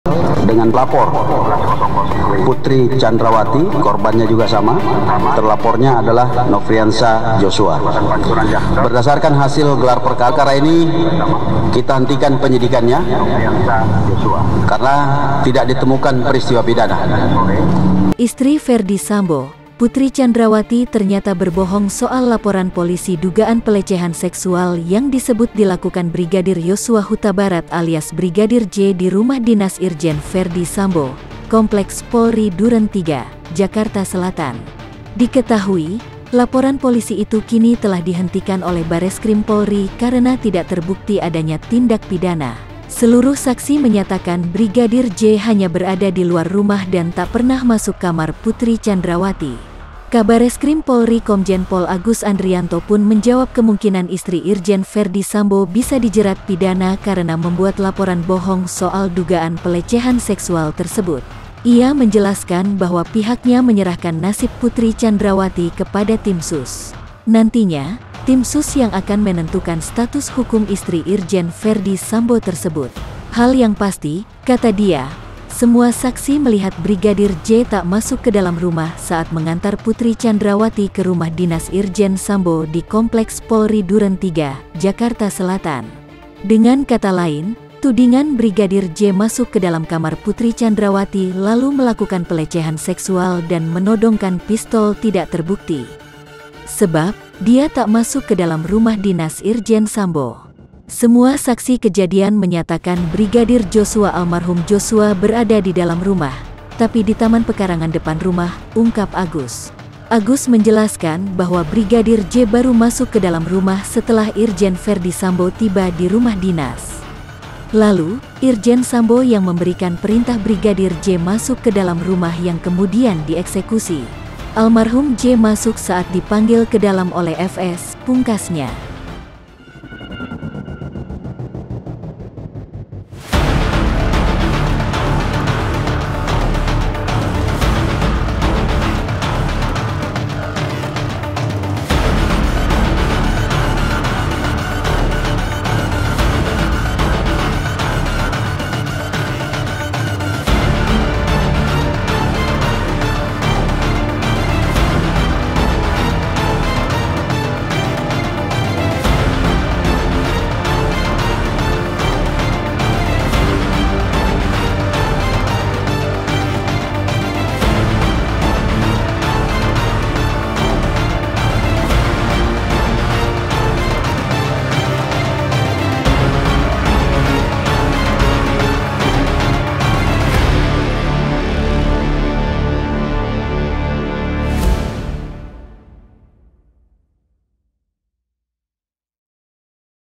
Putri Candrawathi Berbohong Tentang Laporan Polisi Dugaan Pelecehan, Kemungkinan Bisa Dijerat Pidana
POS-KUPANG.COM - Istri Ferdy Sambo, Putri Candrawathi ternyata berbohong soal laporan polisi dugaan pelecehan seksual yang disebut dilakukan Brigadir Yosua Hutabarat alias Brigadir J di rumah dinas Irjen Ferdy Sambo, Kompleks Polri Duren Tiga, Jakarta Selatan.

Kabareskrim Polri Komjen Pol Agus Andrianto pun menjawab kemungkinan Istri Irjen Ferdy Sambo bisa dijerat pidana karena membuat laporan bohong soal dugaan pelecehan seksual tersebut.Minggu 14 Agustus 2022 (POS-KUPANG.COM,tribunnews)

VPO             : Jocc